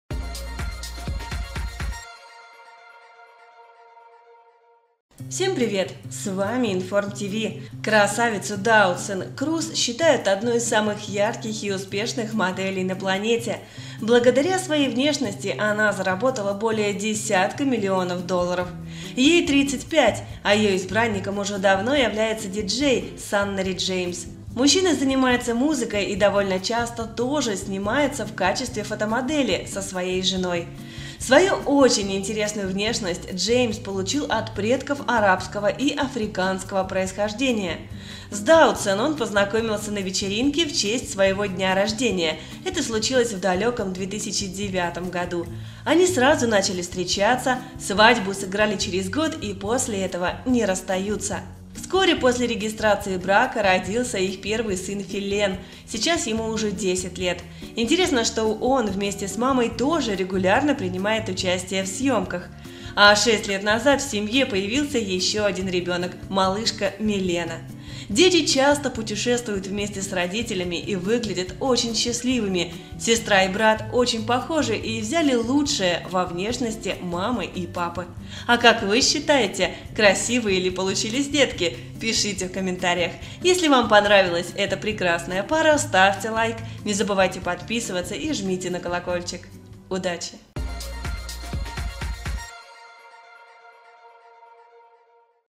НЕ УПАДИТЕ! Как выглядят дети самой красивой пары в мире
Красавицу ДАутсен Крус считают одной самых ярких и успешных моделей на планете. Благодаря своей внешности она заработала более десятка миллионов долларов.

Ей 35 лет и ее избранником уже давно является диджей Саннери Джеймс.  Занимается музыкой и довольно часто тоже снимается в качестве фотомодели со своей женой. Свою очень интересную внешность Джеймс получил от предков арабского и африканского происхождения.

С Даутсен он познакомился на вечеринке в честь своего дня рождения в 2009 году. И они сразу начали встречаться, но свадьбу сыграли через год.

Вскоре родился первый сын Филлен, ему сейчас уже 10 лет. Интересно, что он вместе с мамой тоже регулярно принимает участие в съемках.

А 6 лет назад в семье появился еще один ребенка – малышка Милена

Дети постоянно путешествуют вместе с родителями и выглядят очень счастливыми. Сестра и брат очень похожи и взяли лучшее во внешности своих родителей.